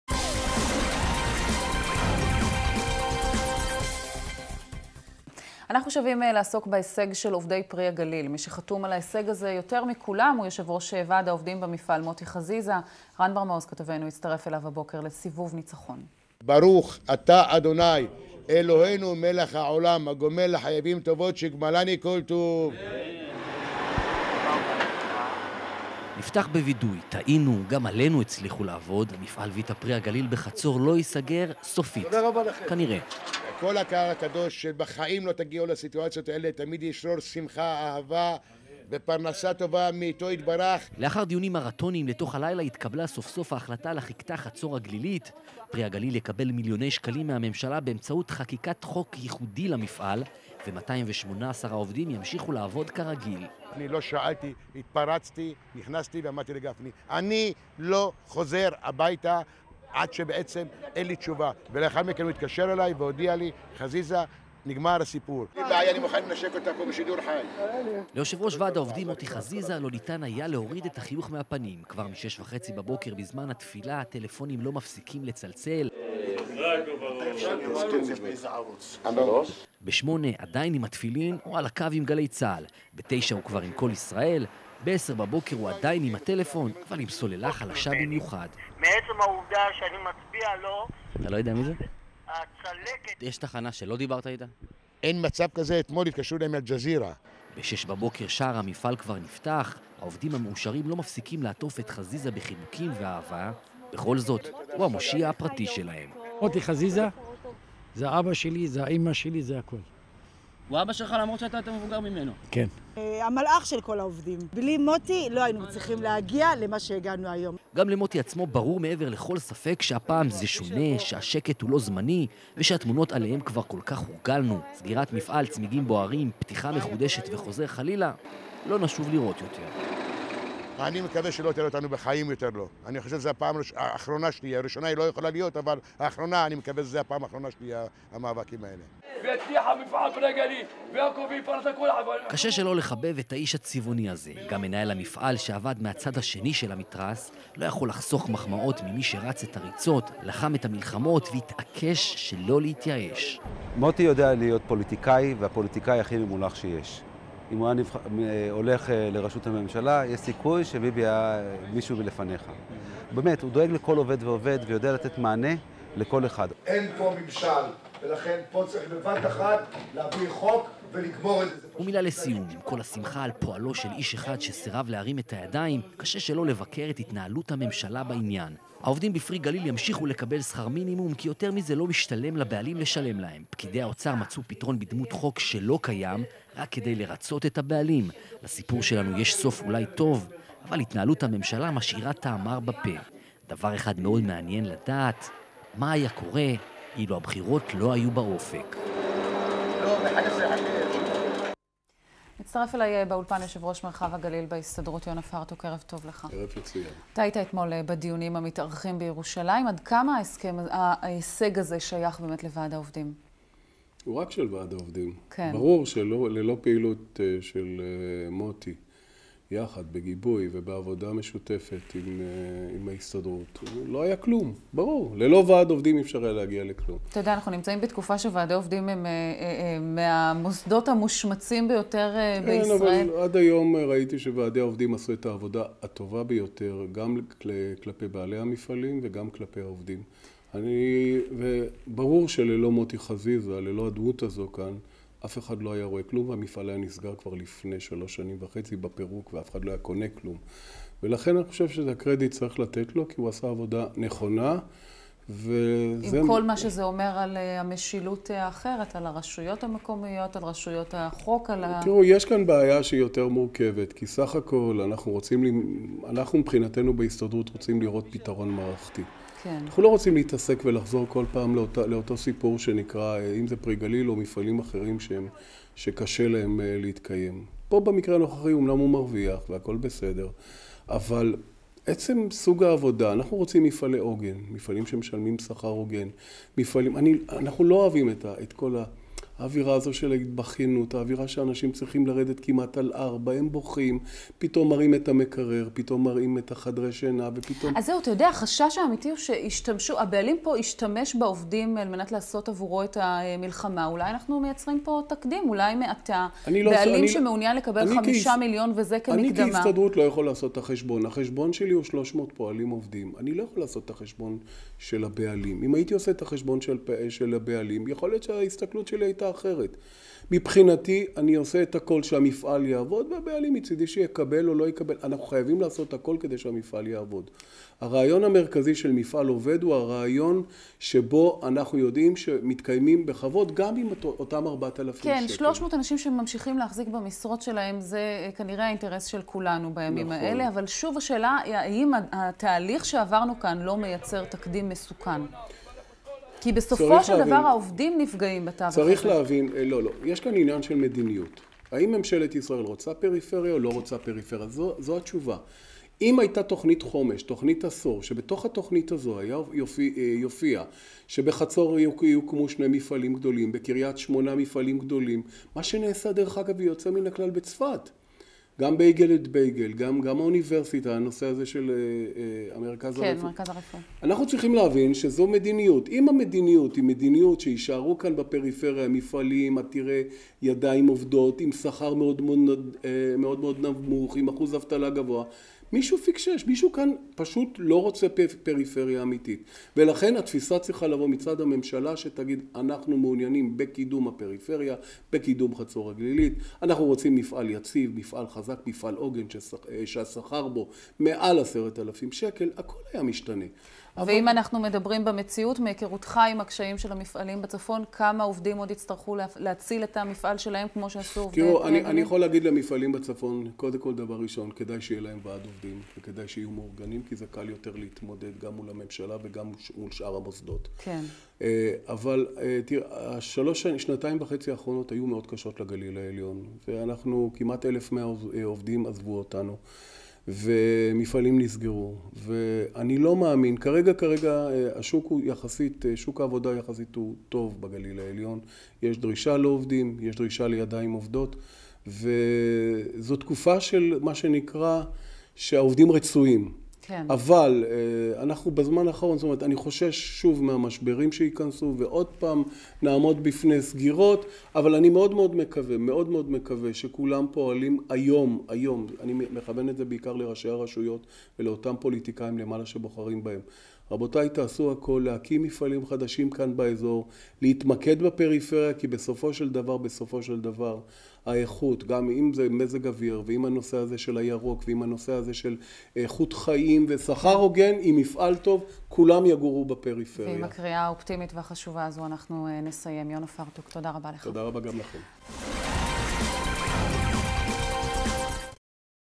סיבוב הניצחון
יו"ר ועד העובדים במפעל פרי הגליל בחצור חוגג עם העובדים חזרה לשגרה. כתבה של רן בר מעוז, וראיון אולפן עם יונה פרטוק יו"ר מרחב הצפון בהסתדרות